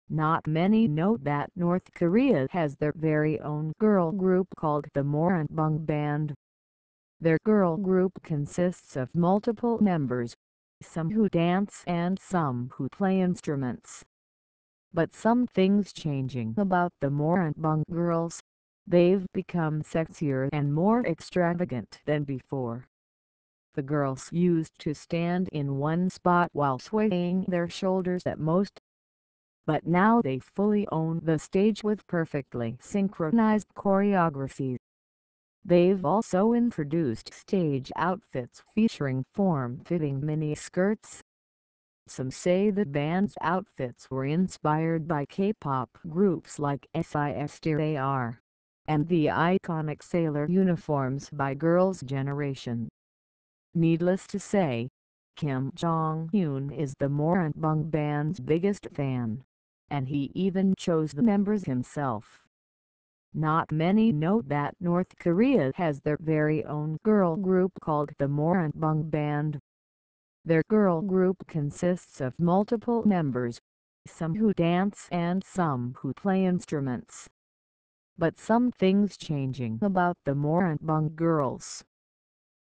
Meet The Moranbong Band, North Korea’s Girls’ Generation
Not many know that North Korea has their very own girl group called the Moranbong Band!

Their girl group consists of multiple members, some who dance and some who play instruments.

But something’s changing about the Moranbong girls…

… they’ve become sexier and more extravagant than before!

The girls used to stand in one spot while swaying their shoulders at most.

But now they fully own the stage with perfectly synchronized choreography!

They’ve also introduced stage outfits featuring form-fitting mini skirts!

Some say the band’s outfits were inspired by K-Pop groups like SISTAR…

… and the iconic sailor uniforms by Girls’ Generation!

Needless to say, Kim Jong Eun is the Moranbong Band’s biggest fan, and he even chose the members himself!